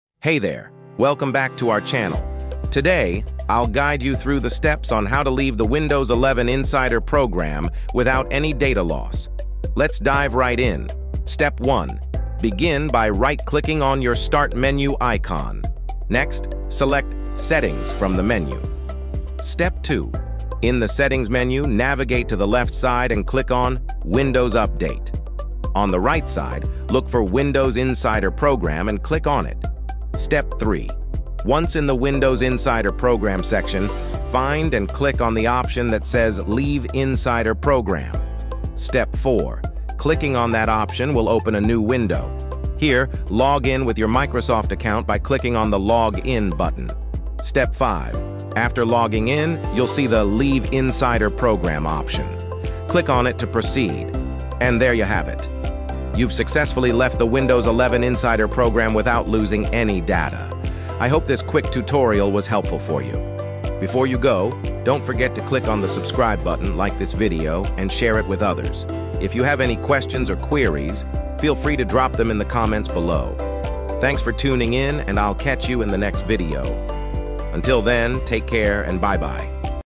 Leaving WINDOWS 11 INSIDER Program Without Losing Your Data (2024)
Leaving Windows 11 Insider Program Safely Without Losing Your Data

Discover the hassle-free way to exit the Windows 11 Insider Program without risking your data loss. Follow our step-by-step guide for a smooth transition.

Steps :-

**Leaving Windows 11 Insider Program: Step-by-Step Guide**

1. **Access Settings:**
   - Right-click on the Start menu icon and select "Settings."

2. **Navigate to Windows Update:**
   - Inside the Settings menu, go to the left side and click on "Windows Update."

3. **Enter Windows Insider Program:**
   - Locate "Windows Insider Program" on the right side and click on it.

4. **Leave Insider Program:**
   - In the Windows Insider Program section, find and click on "Leave Insider Program."

5. **Log In with Microsoft Account:**
   - A new window will open; log in with your Microsoft account by clicking the Log In button.

6. **Finalize Leaving Insider Program:**
   - After logging in, find the "Leave Insider Program" option and click on it.

7. **Confirmation:**
   - Confirm your choice to leave the program.

8. **Data Integrity Preserved:**
   - You've successfully left the Windows 11 Insider Program without losing any data.

9. **Engage with the Channel:**
   - Drop any questions or comments below the video.